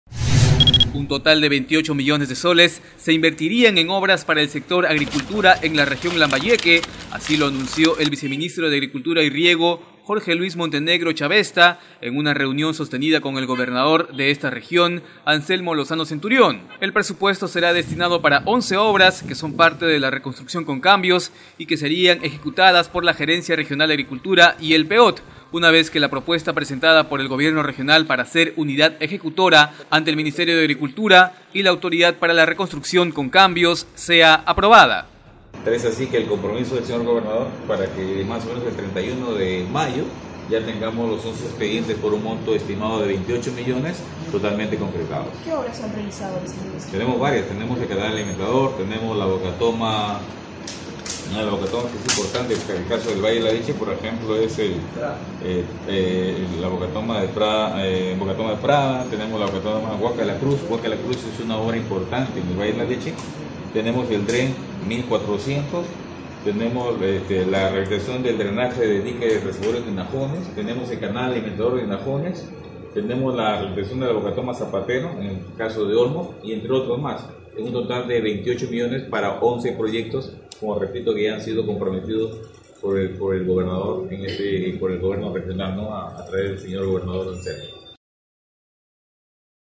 Chiclayo: 28 millones para obras en sector agricultura
Se ha anunciado un presupuesto aprobado de 28 millones de soles para ser invertidos en el sector agricultura en nuestra región. La información fue corroborada por el Viceministro de Agricultura y Riego, Jorge Luis Montenegro Chavesta, en una reunión sostenida con el gobernador de Lambayeque.